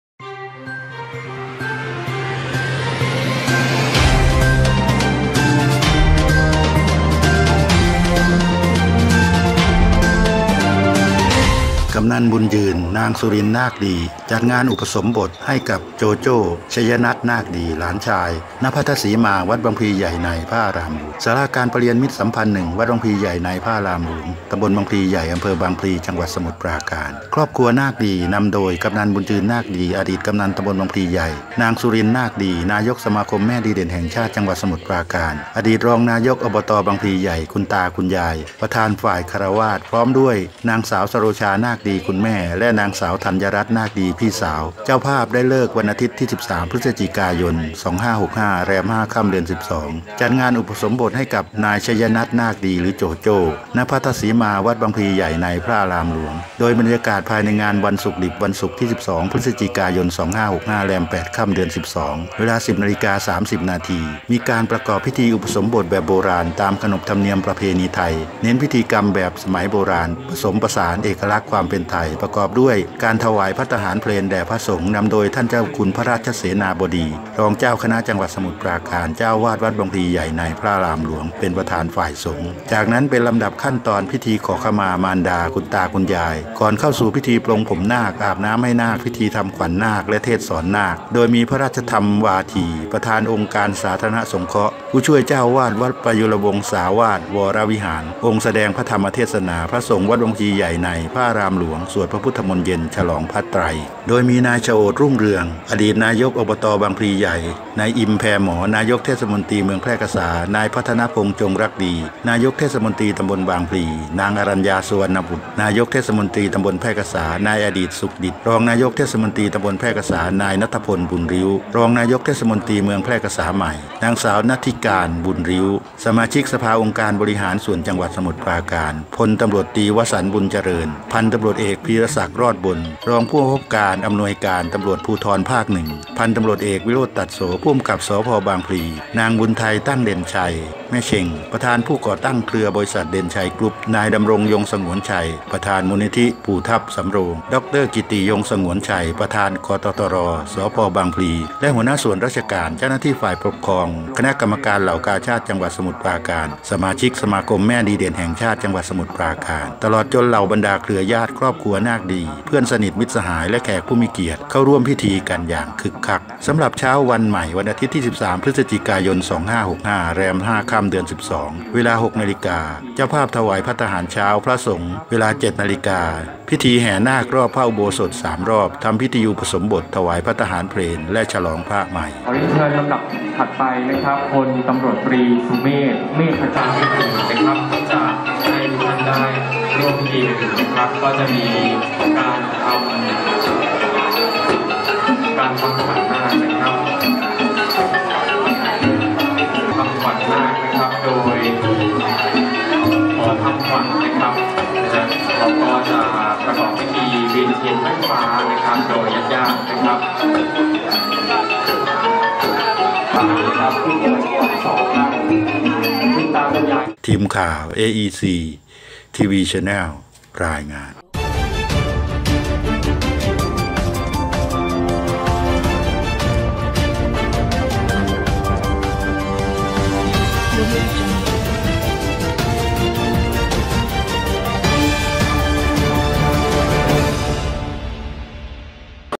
กำนันบุญยืน นางสุรินทร์ นาคดี จัดงานอุปสมบท ให้กับ โจโจ้ ชยนัฏฐ์ นาคดี หลานชาย ณ พัทธสีมาวัดบางพลีให
กำนันบุญยืน นางสุรินทร์ นาคดี จัดงานอุปสมบท ให้กับ โจโจ้ ชยนัฏฐ์ นาคดี (หลานชาย) ณ พัทธสีมาวัดบางพลีใหญ่ใน พระอารามหลวง tvonline tvstreaming #
ดูทีวีสดผ่านมือถือที่นี่ #

          วันที่ 12 พฤศจิกายน 2565 ที่ ศาลาการเปรียญ มิตรสัมพันธ์ 1 วัดบางพลีใหญ่ใน พระอารามหลวง ต.บางพลีใหญ่ อ.บางพลี จ.สมุทรปราการ ครอบครัวนาคดี นำโดย กำนันบุญยืน นาคดี อดีตกำนันตำบลบางพลีใหญ่ นางสุรินทร์ นาคดี นายกสมาคมแม่ดีเด่นแห่งชาติจังหวัดสมุทรปราการ อดีตรองนายก อบต.บางพลีใหญ่ (คุณตา-คุณยาย) ประธานฝ่ายฆราวาส พร้อมด้วย น.ส.สโรชา นาคดี (คุณแม่) และน.ส.ธัญยรัชต์ นาคดี (พี่สาว) เจ้าภาพ ได้ฤกษ์ วันอาทิตย์ที่ 13 พฤศจิกายน 2565 (แรม 5 ค่ำ เดือน 12 )จัดงานอุปสมบท ให้กับ นายชยนัฏฐ์ นาคดี หรือ โจโจ้ ณ พัทธสีมาวัดบางพลีใหญ่ใน พระอารามหลวง โดยบรรยากาศภายในงาน วันสุกดิบ วันศุกร์ที่ 12 พฤศจิกายน 2565 แรม 8 ค่ำ เดือน 12 เวลา 10.30 น.มีการประกอบพิธีอุปสมบทแบบโบราณตามขนบธรรมเนียมประเพณีไทยเน้นพิธีกรรมแบบสมัยโบราณ ประสมประสานเอกลักษณ์ความเป็นไทย ประกอบด้วย การถวายภัตตาหารเพล แด่พระสงฆ์ นำโดย ท่านเจ้าคุณพระราชเสนาบดี รองเจ้าคณะจังหวัดสมุทรปราการ เจ้าอาวาสวัดบางพลีใหญ่ใน พระอารามหลวง เป็นประธานฝ่ายสงฆ์ จากนั้น เป็นลำดับขั้นตอน พิธีขอขมา มารดา คุณตา-คุณยาย ก่อนเข้าสู่พิธีปลงผมนาค อาบน้ำนาค พิธีทำขวัญนาค และเทศน์สอนนาค โดยมี พระราชธรรมวาที ประธานองค์การสาธารณสงเคราะห์ ผู้ช่วยเจ้าอาวาสวัดประยุรวงศาวาสวรวิหาร.องค์แสดงพระธรรมเทศนา พระสงฆ์วัดบางพลีใหญ่ใน พระอารามหลวง สวดพระพุทธมนต์เย็น ฉลองผ้าไตรฯ
         โดยมี นายฉะโอด รุ่งเรือง อดีตนายก อบต.บางพลีใหญ่ นายอิม แพหมอ นายกเทศมนตรีเมืองแพรกษา นายพัฒนพงศ์ จงรักดี นายกเทศมนตรีตำบลบางพลี นางอรัญญา สุวรรณบุตร นายกเทศมนตรีตำบลแพรกษา นายอดีต สุกดิษฐ์ รองนายกเทศมนตรีตำบลแพรกษา นายณัฐพล บุญริ้ว รอง นายกเทศมนตรีเมืองแพรกษาใหม่ น.ส.มณิฐกานต์ บุญริ้ว สมาชิกสภาองค์การบริหารส่วนจังหวัดสมุทรปราการ พล.ต.ต.วสันต์ บุญเจริญ พ.ต.อ.พีรศักดิ์ รอดบน รอง.ผบก.อก.ตำรวจภูธรภาค.1 พ.ต.อ.วิโรจน์ ตัดโส ผกก.สภ.บางพลี นางบุญไทย ตั้งเด่นไชย (แม่เชง)ประธานผู้ก่อตั้งเครือบริษัท เด่นชัยกรุ๊ป นายดำรงค์ ยงค์สงวนชัย ประธานมูลนิธิปู่ทัพสำโรง ดรร.กิตติ ยงค์สงวนชัย ประธาน (กต.ตร.) สภ.บางพลี  และ หัวหน้าส่วนราชการ เจ้าหน้าที่ฝ่ายปกครอง คณะกรรมการเหล่ากาชาดจังหวัดสมุทรปราการ สมาชิกสมาคมแม่ดีเด่นแห่งชาติจังหวัดสมุทรปราการ ตลอดจน เหล่าบรรดาเครือญาติ ครอบครัวนาคดี เพื่อนสนิท มิตร สหาย และแขกผู้มีเกียรติ เข้าร่วมพิธีกันอย่างคึกคัก